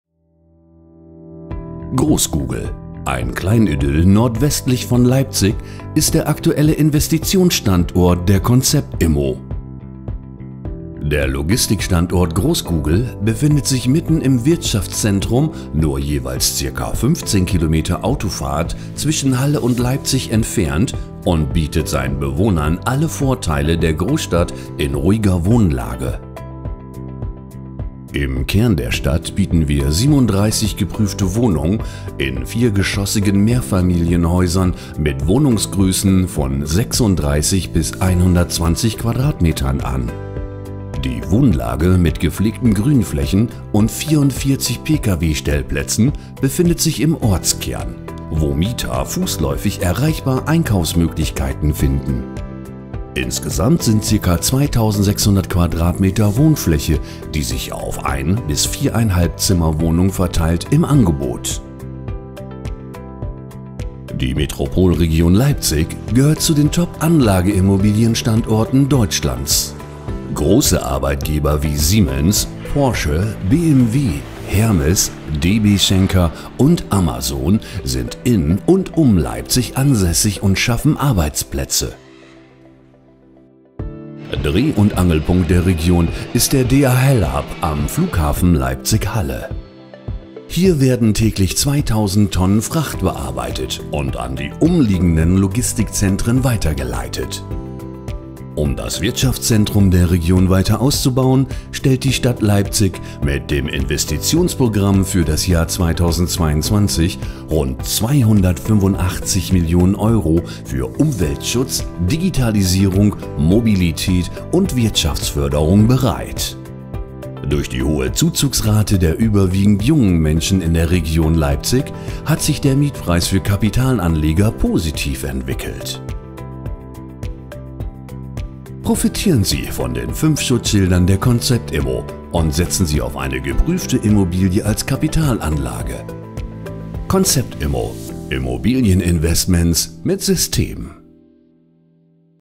Projektfilm Großkugel - Kleinidyll westlich von Leipzig
Großkugel - ein Kleinidyll westlich von Leipzig ist unser aktueller Investitionsstandort der KonzeptImmo.

Der Logistikstandort Großkugel befindet sich mitten im Wirtschaftszentrum nur jeweils ca. 15 Kilometer Autofahrt zwischen Halle und Leipzig und bietet seinen Bewohnern alle Vorteile der Großstadt in ruhiger Wohnlage.

Im Kern der Stadt bieten wir 37 geprüfte Wohnungen in viergeschossigen Mehrfamilienhäusern mit Wohnungsgrößen von 36 bis 120 qm an.

Die Wohnanlage mit gepflegten Grünflächen und 44 Pkw Stellplätzen befinden im Ortskern, wo Mieter fußläufig erreichbar Einkaufsmöglichkeiten finden. Insgesamt sind ca. 2600 qm Wohnfläche, die sich auf 1 - 4 Zimmer-Wohnungen verteilt, im Angebot.

Die Metropolregion Leipzig gehört zu den top Anlageimmobilienstandorten Deutschlands. Große Arbeitgeber wie Siemens, Porsche, BMW, Hermes, DB Schenker und Amazon sind in und um Leipzig ansässig und schaffen Arbeitsplätze. Dreh- und Angelpunkt der Region ist der DHL-Hub am Flughafen-Leipzig/Halle - hier werden täglich über 2000 Tonnen Fracht bearbeitet und an die umliegenden Logistikzentren weiterverteilt.

Um das Wirtschaftszentrum der Region weiter auszubauen, stellt die Stadt Leipzig mit dem Investitionsprogramm für das Jahr 2022 rund 285 Millionen Euro für Umweltschutz, Digitalisierung, Mobilität und Wirtschaftsförderung bereit.

Durch die hohe Zuzugsrate der überwiegend jungen Menschen in die Region Leipzig hat sich der Mietpreis für Kapitalanleger positiv entwickelt.

Profitieren Sie von den 5 Schutzschildern der KonzeptImmo und setzen Sie auf eine geprüfte Immobilie als Kapitalanlage.